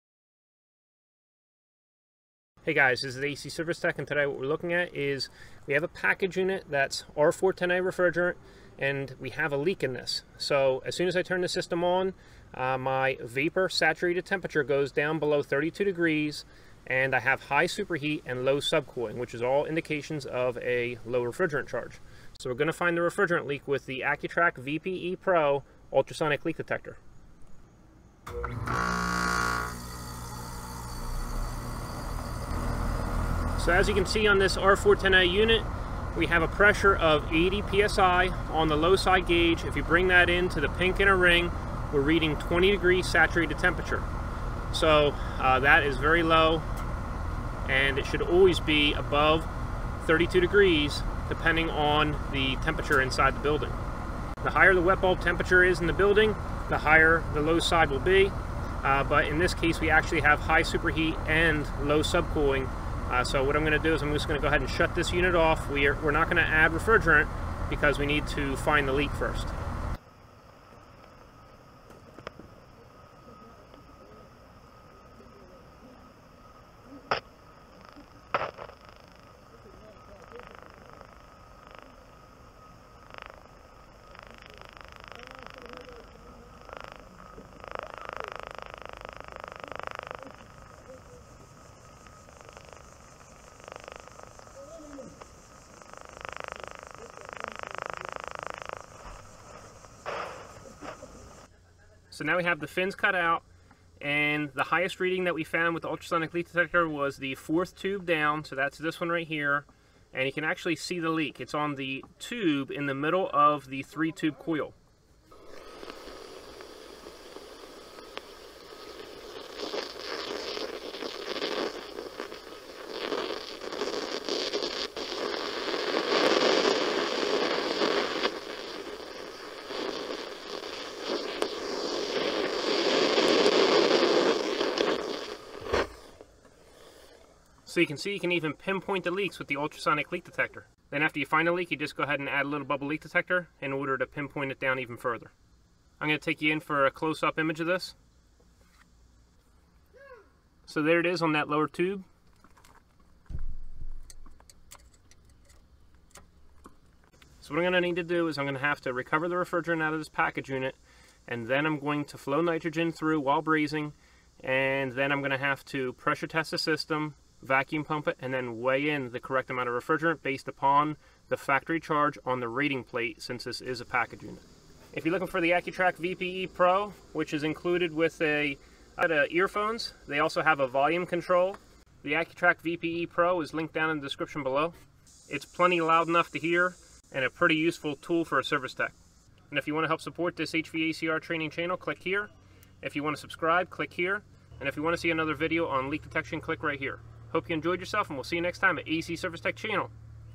How to Find a Refrigerant Leak with an Ultrasonic Leak Detector!
In this HVACR Training Video, I Use the AccuTrak VPE-GN Pro Ultrasonic Leak Detector to Find a very Small Refrigerant Leak in an Evaporator Coil of a Package Unit. Even my Electronic Sniffer could not find it. Afterward, I show an up-close image of the leak. I am glad to be able to rely on this tool! Supervision is needed by a licensed HVACR Tech while doing this as Experience and Apprenticeship garners Wisdom and Safety.

AccuTrak VPE Pro with Headphones
AccuTrak VPE-GN Pro with Headphones